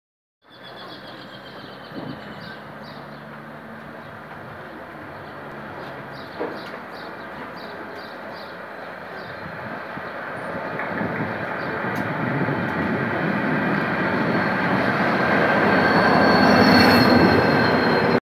GM GT-22 CW-2 A914 maniobrando en Korn, tras llegar con tren de Chascomús
El viernes 7 de enero de 2022 a las 10.54